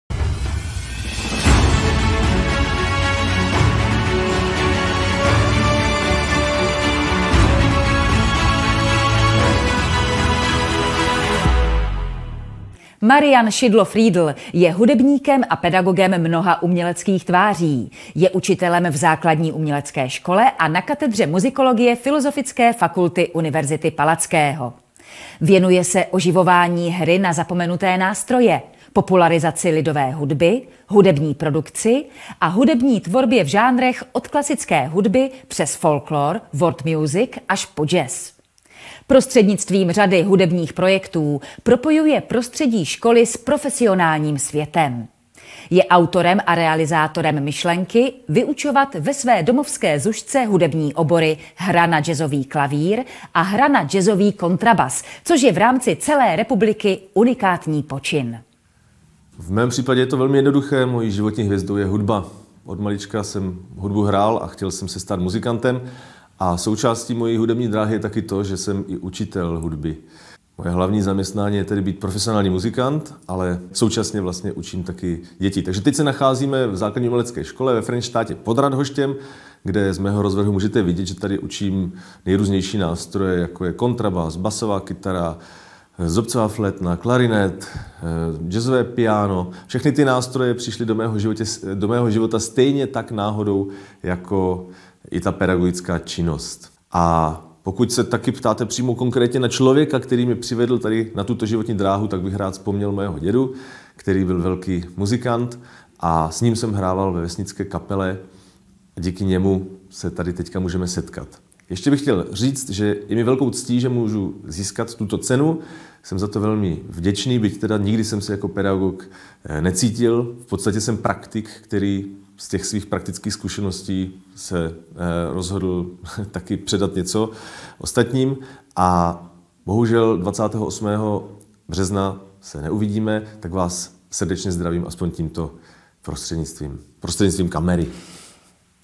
Pedagog roku - Marian Friedl
Náš kolega Marian Friedl byl oceněný spolu s dalšími učiteli v rámci kraje jako výrazná pedagogická osobnost roku. Gratulujeme :-)